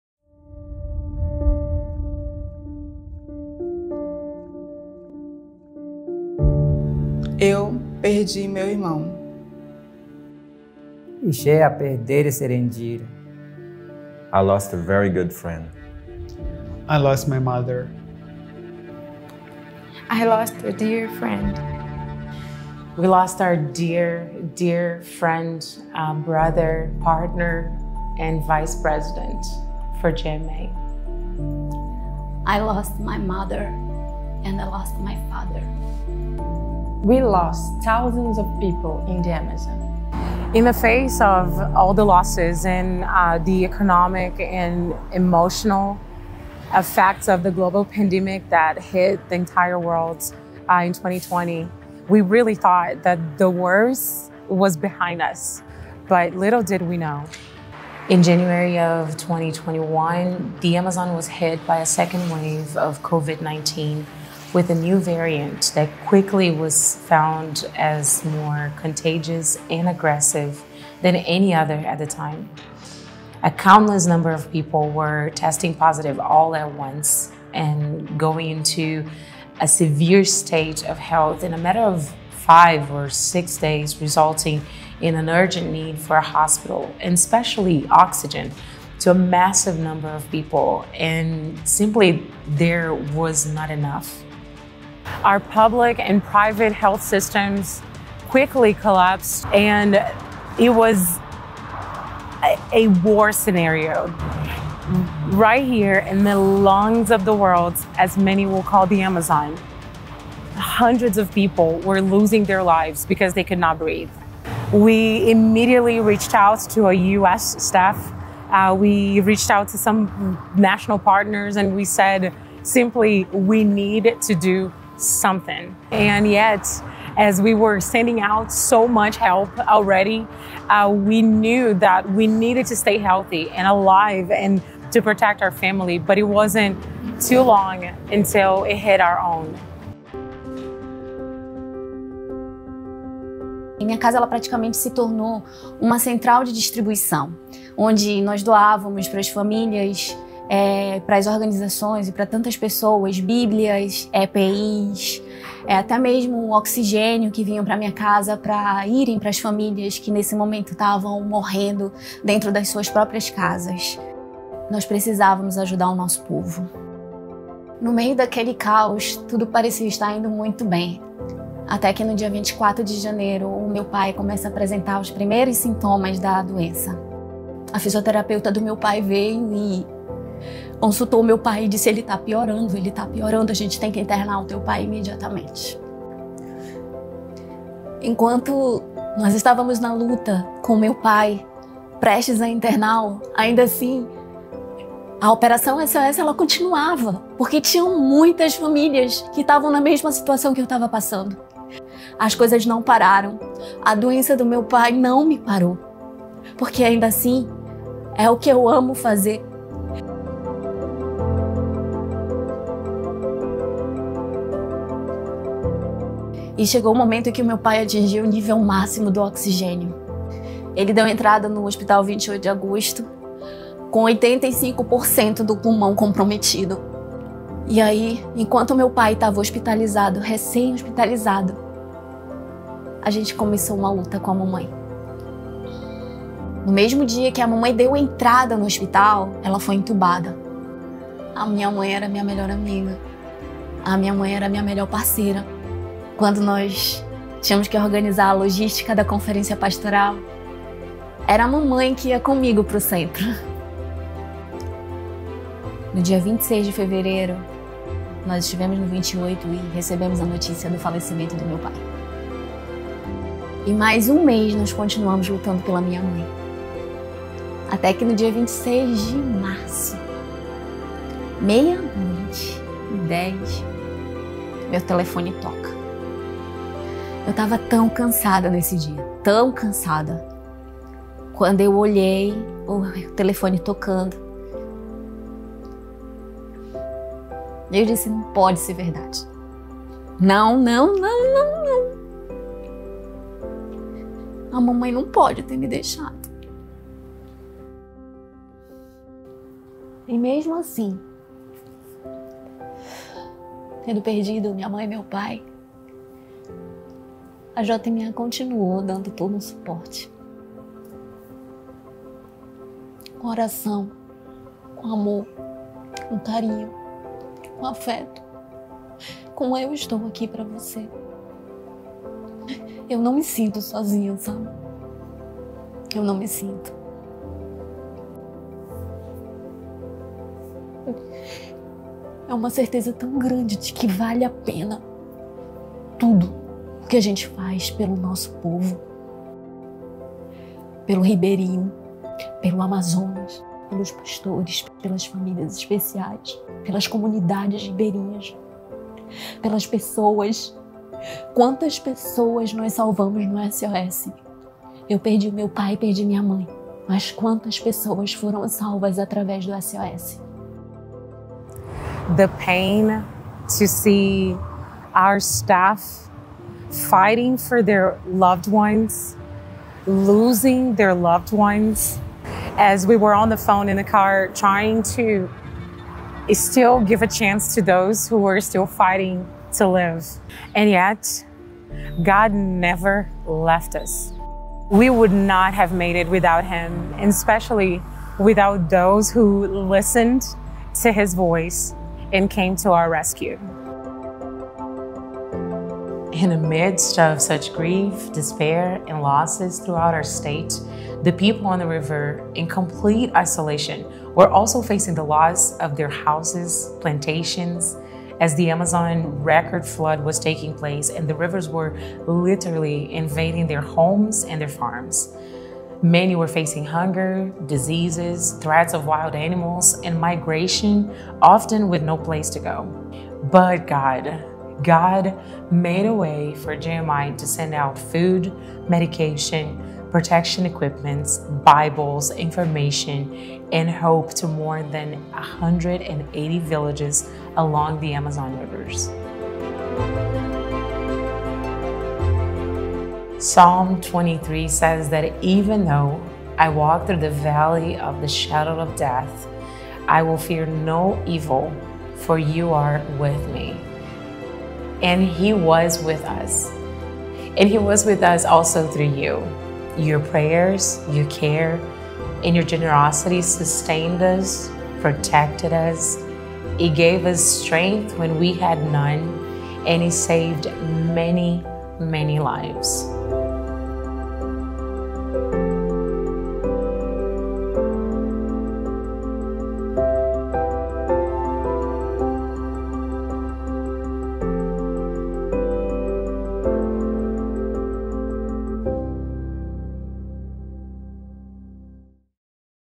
Facing the Pandemic in the Amazon Region of Brazil
COVID hit the state of Amazonas hard in 2020 and 2021. Flooding was at an all-time high and people were desperate for a sign of hope.

JMI's staff on the ground worked tirelessly to bring medicine, oxygen, food, and the love of Christ to those in need. While our staff lost loved ones and grieved for their people, they were able to save so many lives. This is their story.